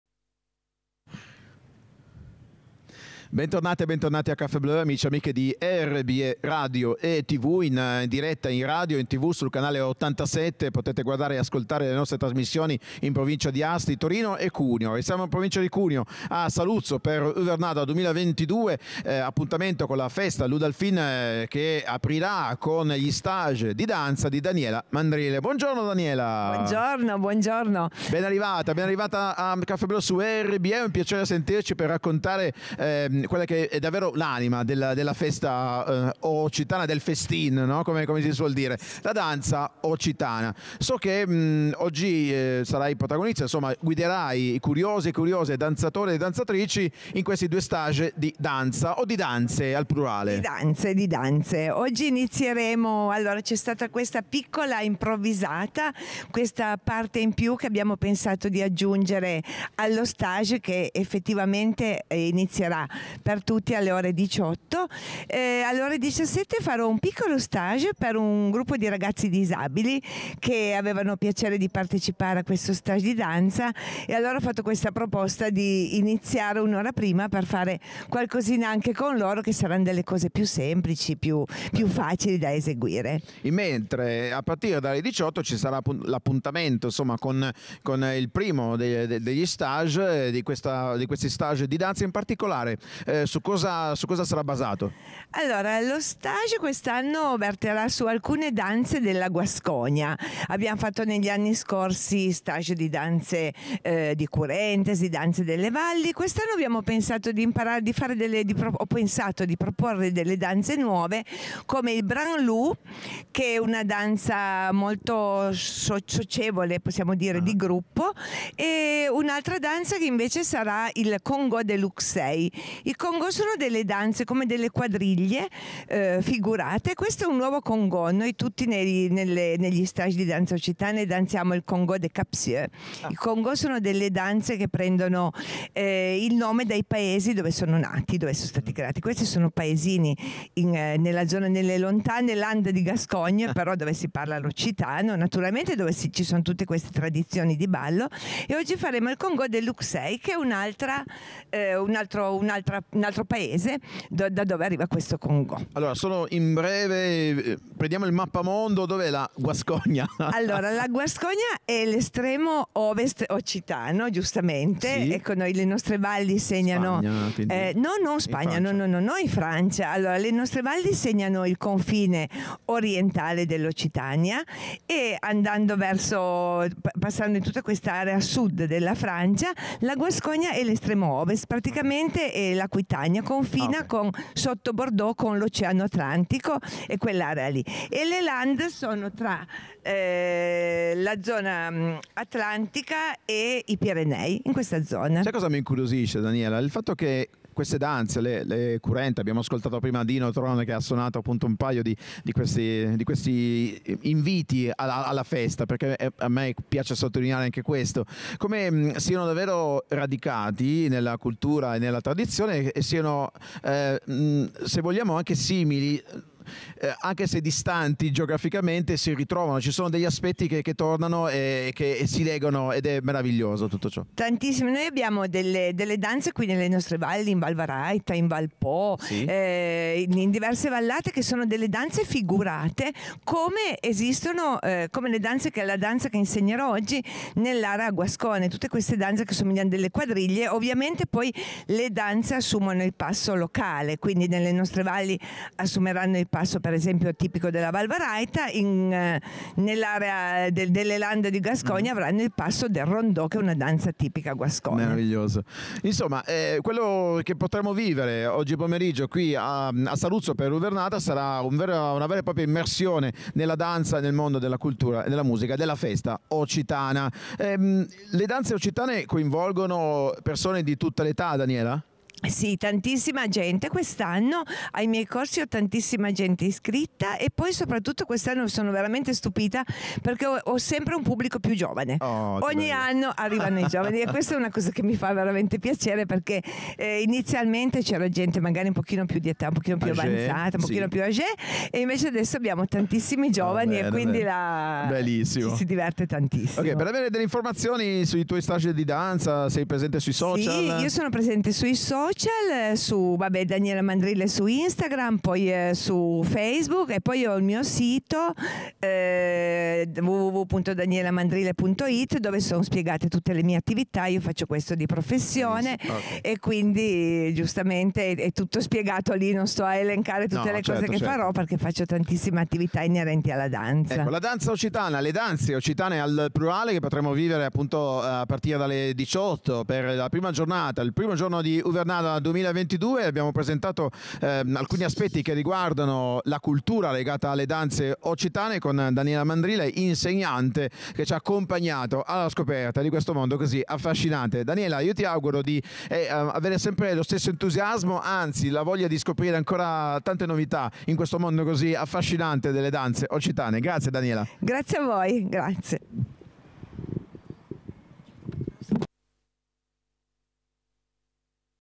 Gli stage di danze occitane a Uvernada 2022 con Daniela Mandrile
piemonte

RBE radio TV, speciale Café Bleu da Saluzzo per Uvernada 2022

Daniela Mandrile, insegnante di danze occitane, presenta gli stage in programma a Saluzzo per Uvernada